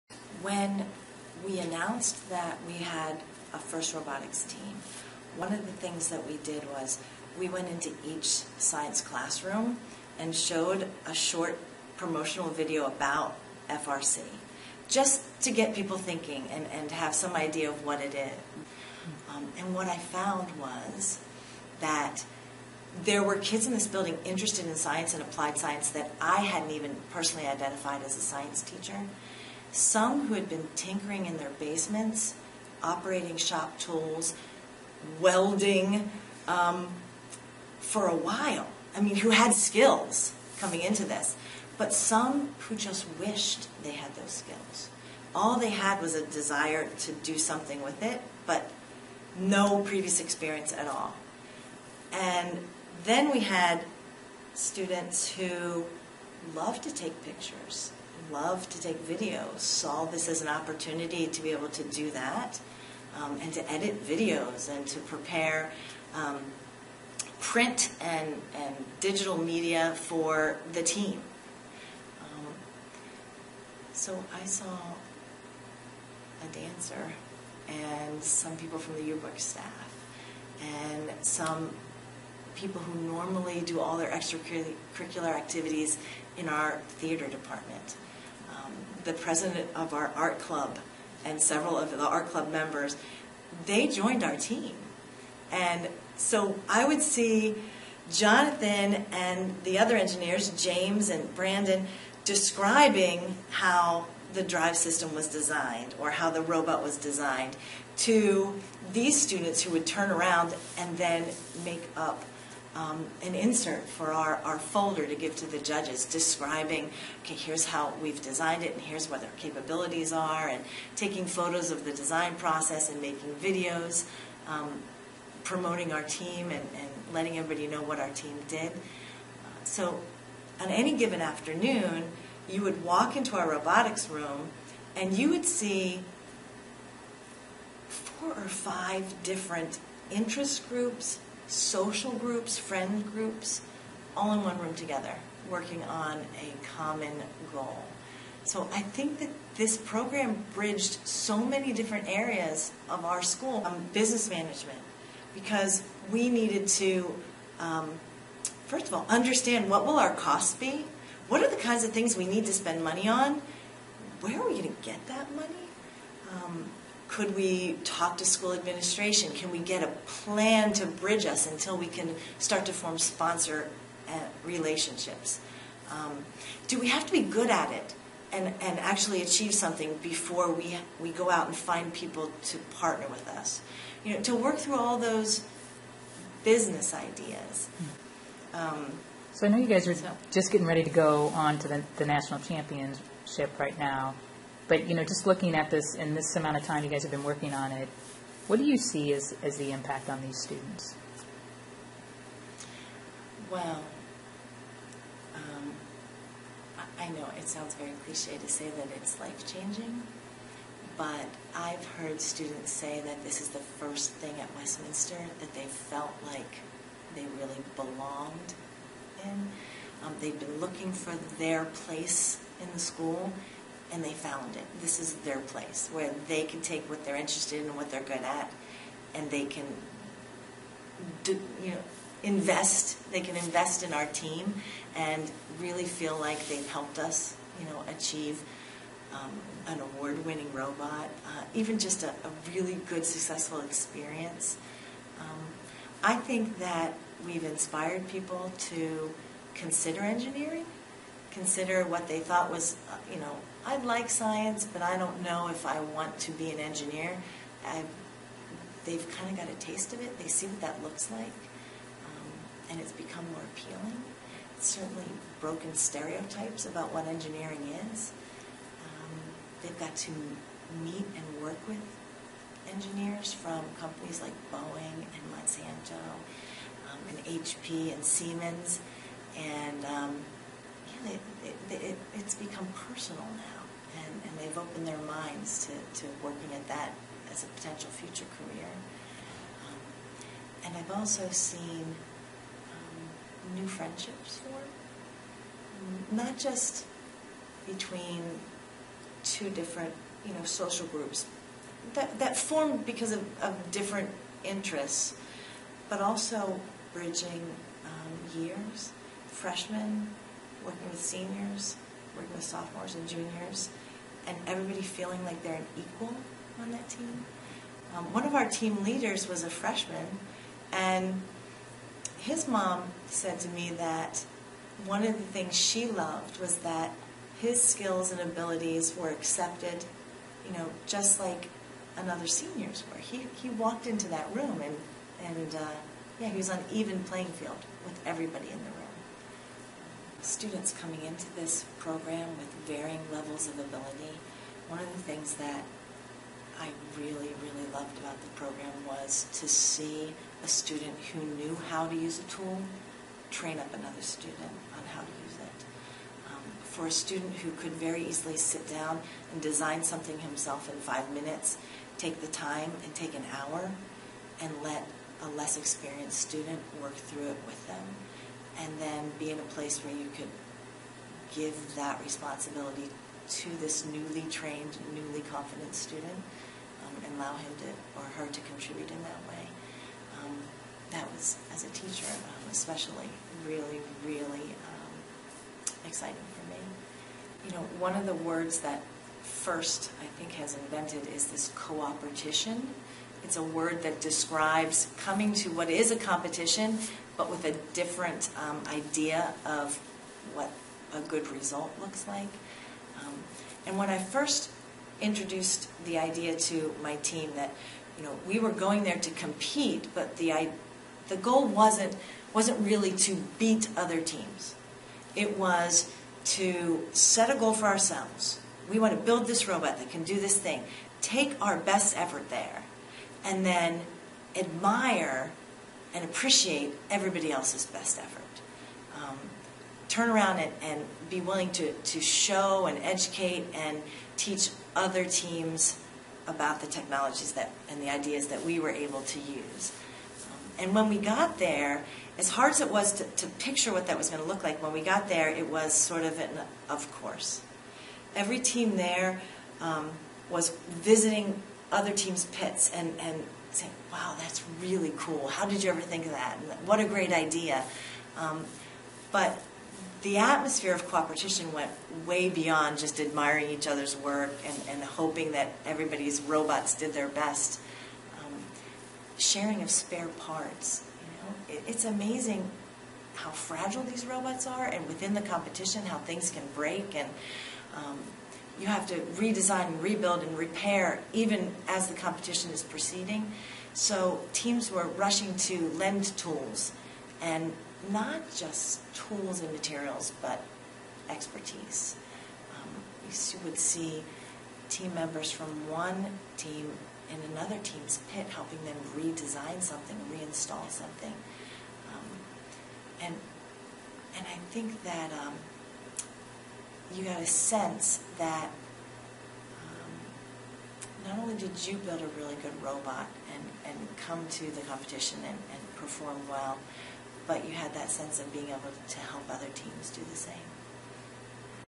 FIRST Robotics Teacher Lisa Harding Part 2
Dora Smith talks with Lisa Harding, science teacher at Westminster Christian Academy, about the school's entry to FIRST Robotics. Westminster won FIRST's regional rookie-of-the-year award. See related blog post